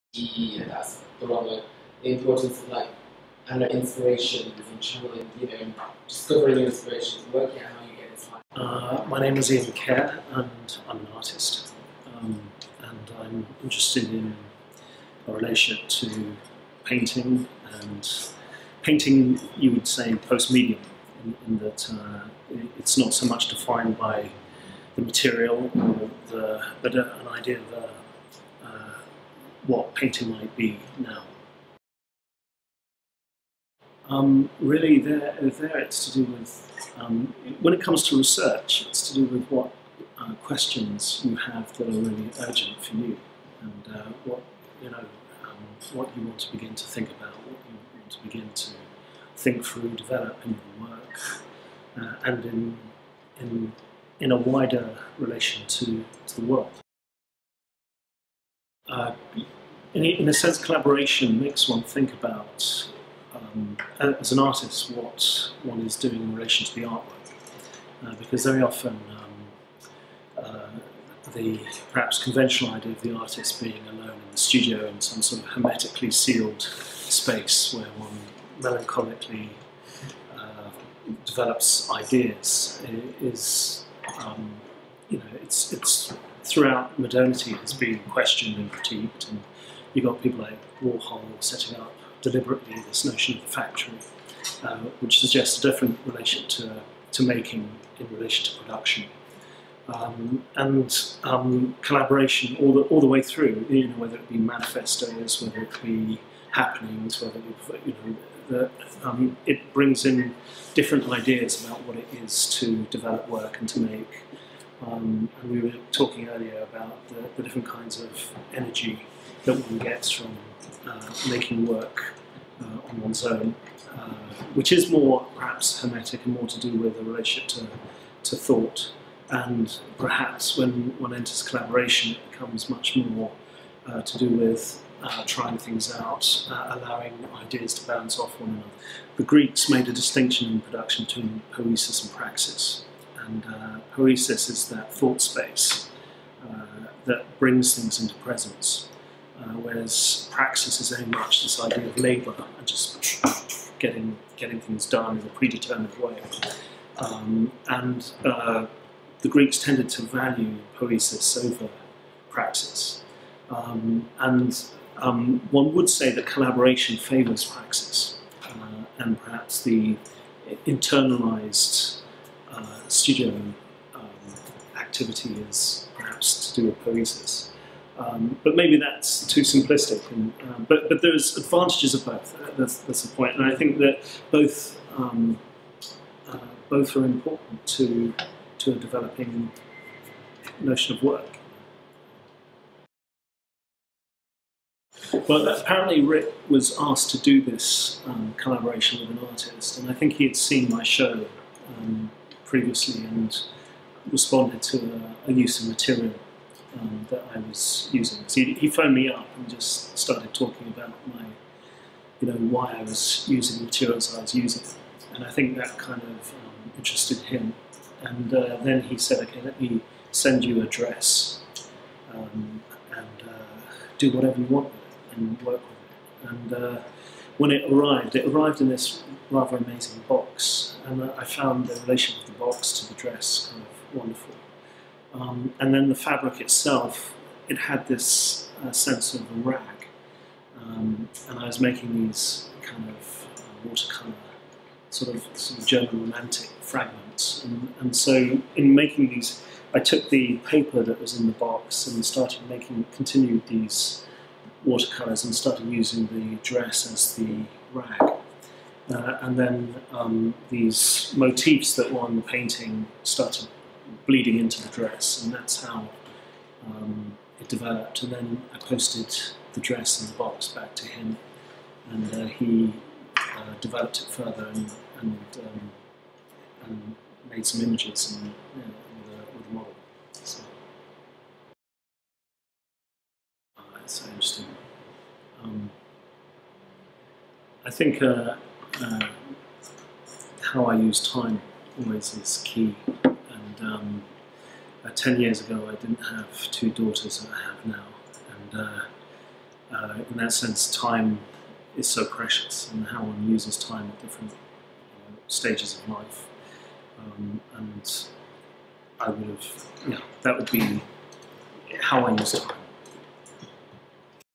NB Collaboration Series Ian Kiaer
Artist Ian Kiaer speaks to NouvelleBox about the value of Collaboration and his own collaboration with Rick Owens